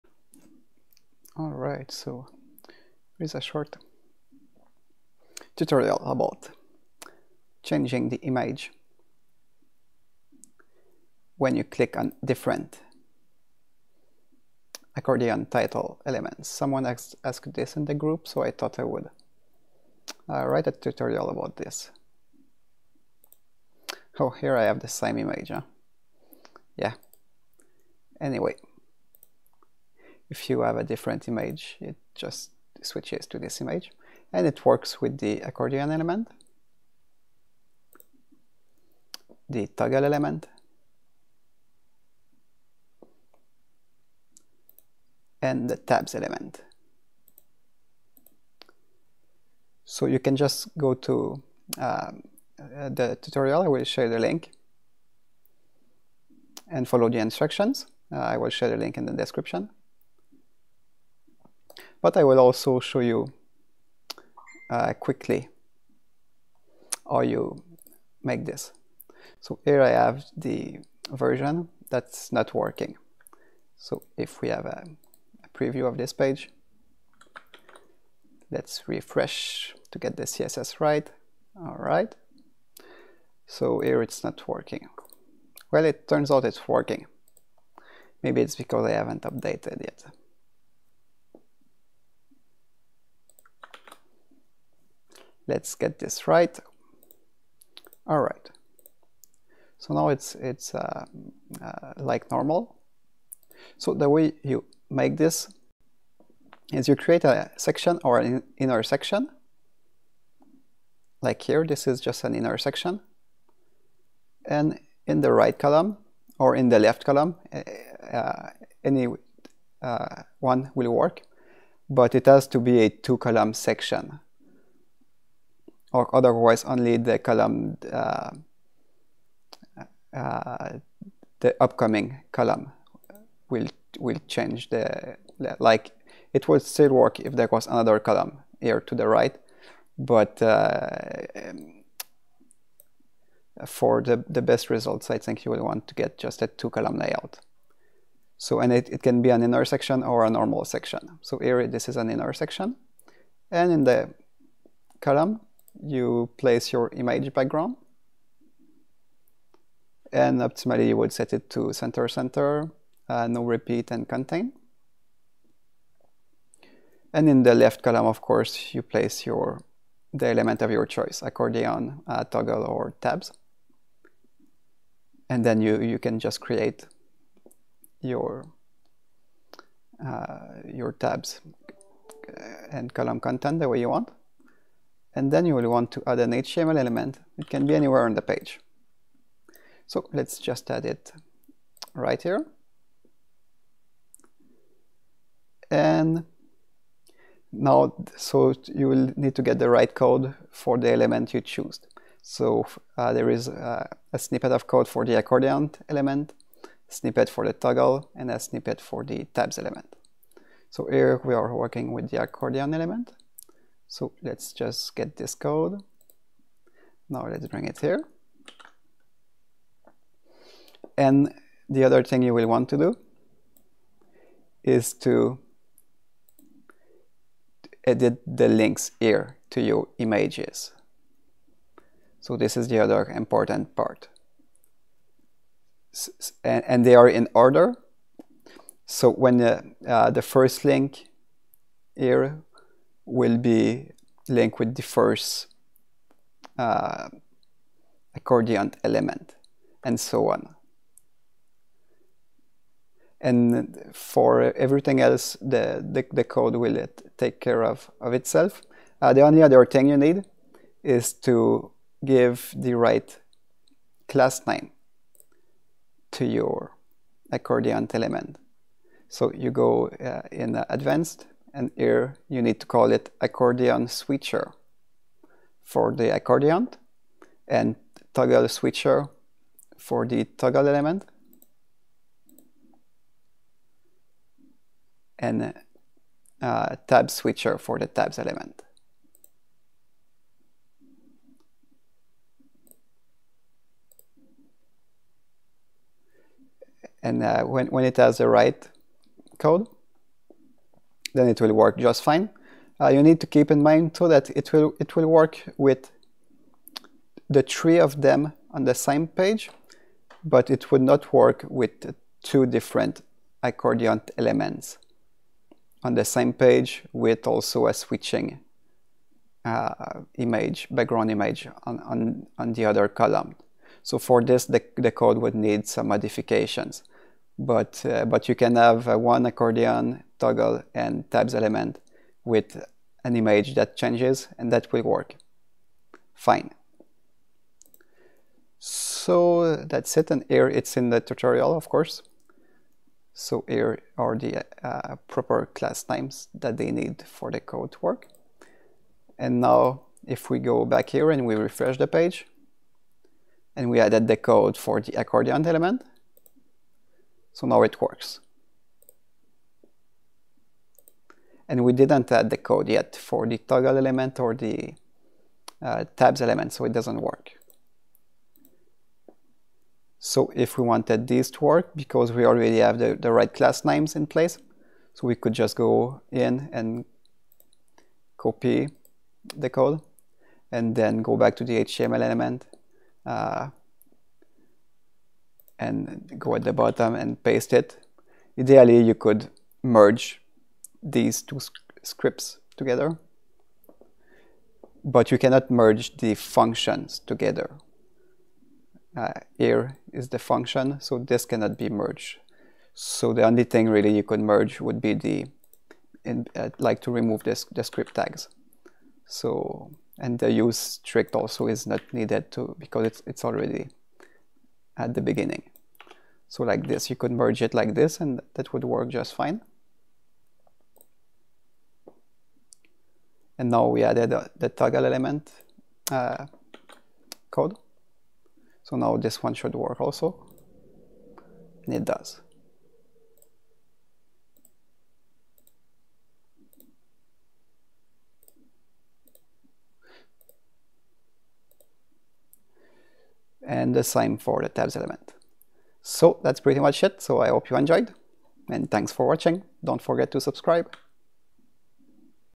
Elementor Accordion, Toggle and Tabs Element Awesome Column Background Images Switcher
Learn how to switch the background of a column upon selecting an accordion, toggle or tabs item! Simple and easy, no extra plugins required!

Consider supporting my work by buying your plugins through my links!